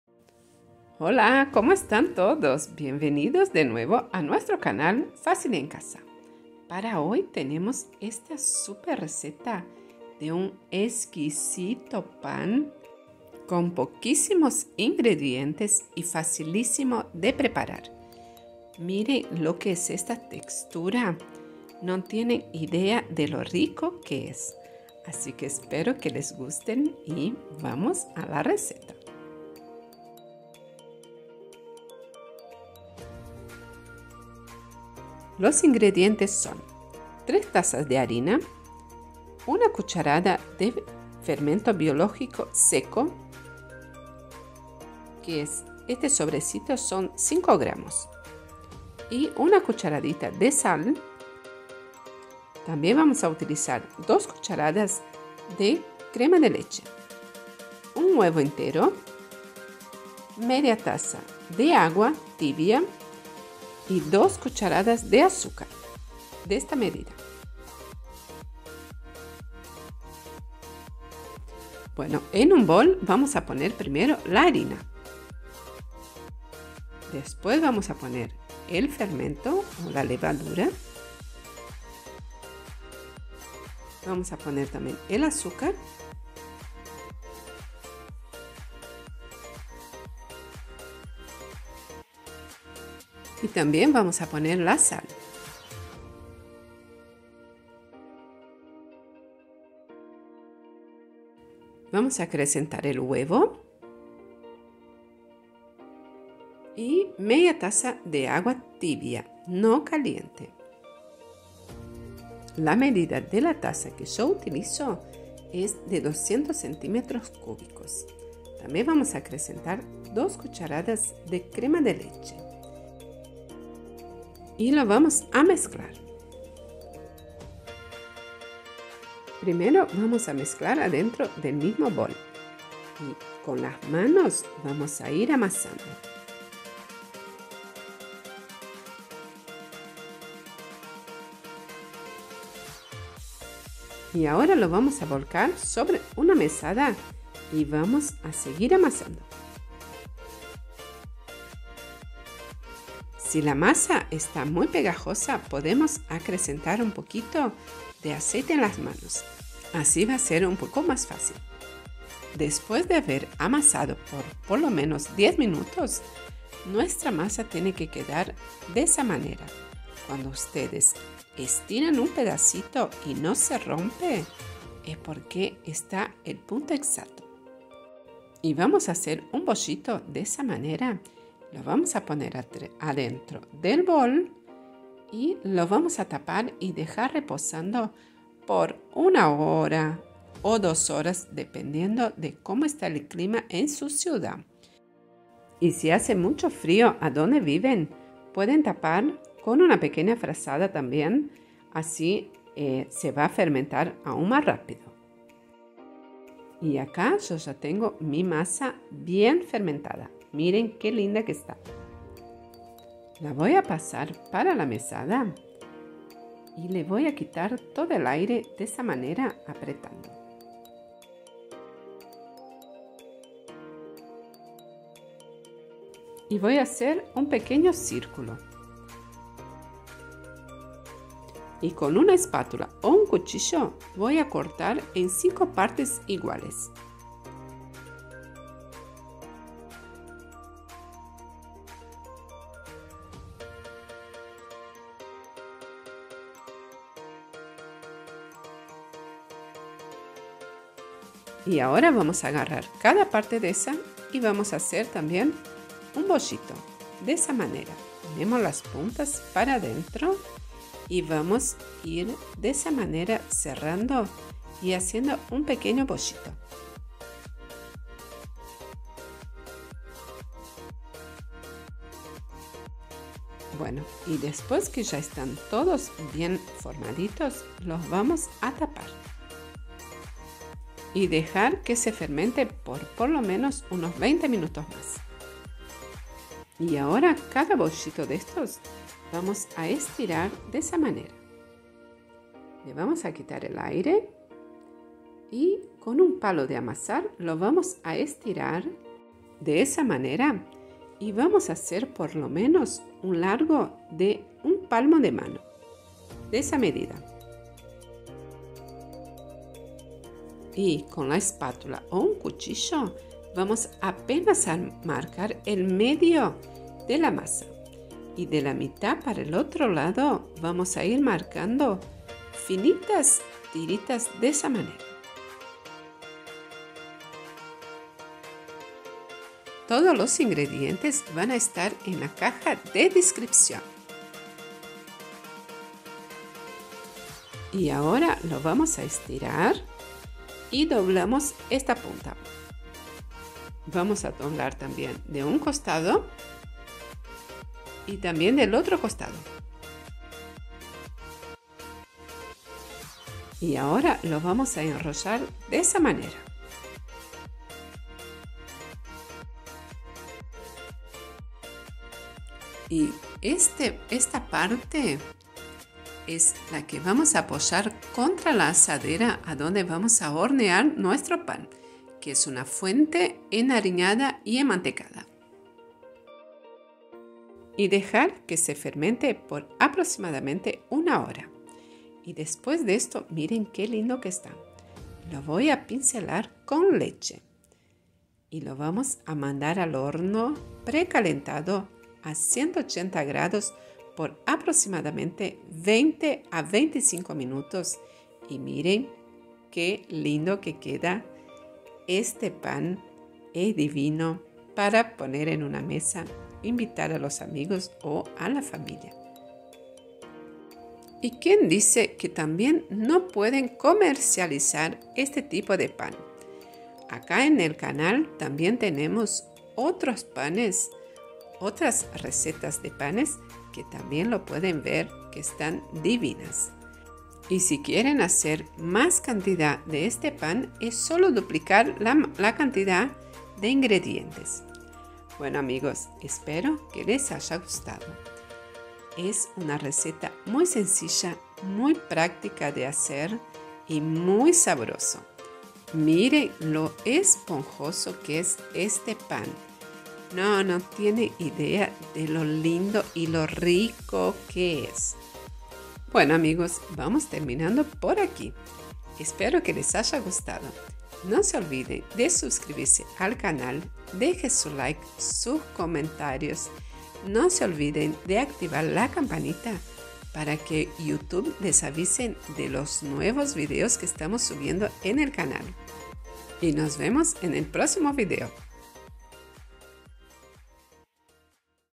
PAN ROLLO DE LANA CASERO / Como hacer de manera fácil y rápida
INGREDIENTES

- 3 tazas de harina
- 2 cucharas de azúcar
- 1 cucharadita de sal
- 1 huevo
- Media taza de agua
- 2 cucharadas de crema de leche
- 5 gramos de levadura seca